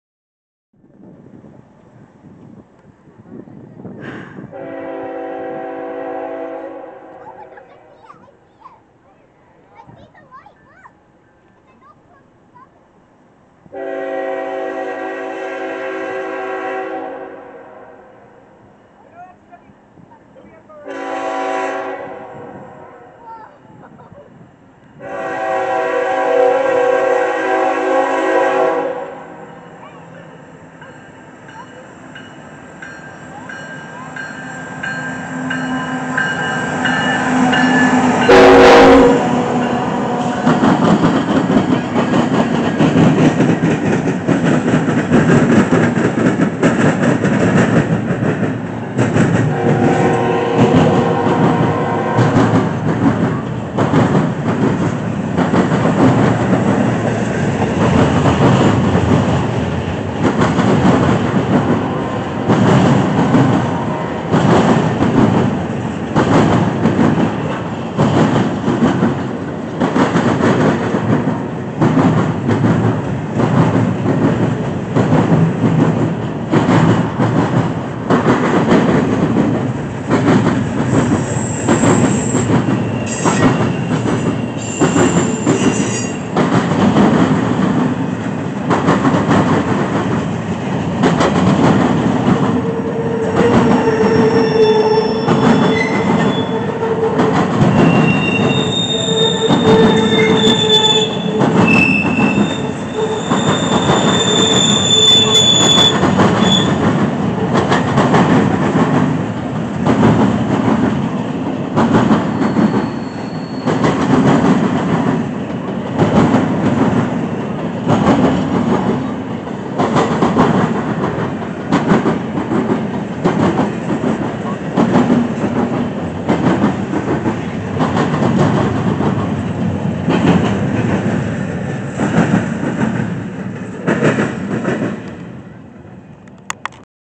Lease Wells Fargo Unit Trailing on an Intermodal
About the Video:
A lease Wells Fargo SD70MAC trailing on an intermodal train in Marion.
Location: Union Station, Marion, Ohio
Filmed: 8/19/23
Camera: HDV-604S
Train Information:
Direction: northbound
Track: NS Sandusky District
Locomotives: 3
Train Cars: 46
Power:
NS 3660 GE ES44AC
NS 1026 EMD SD70ACe
WFRX 8912 EMD SD70MAC